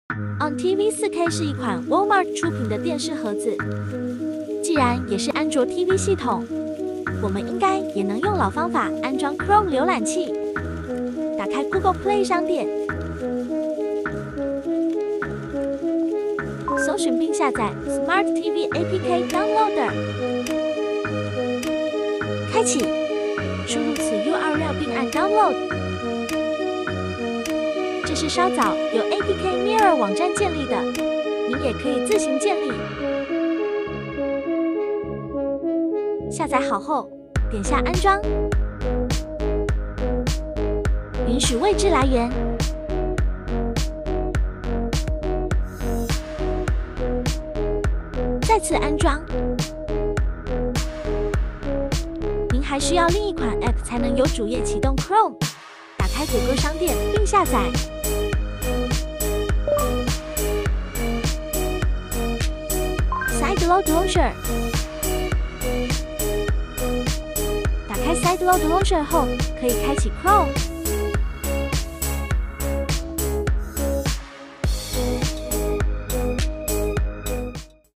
ONN TV 4K PRO 如何安装 CHROME APK
ONN TV 4K 如何在 WALMART ONN TV 4K 上安装谷歌浏览器? 由 GOOGLE PLAY 应用商店中找不到? 没问题，跟着我的教程进行安装。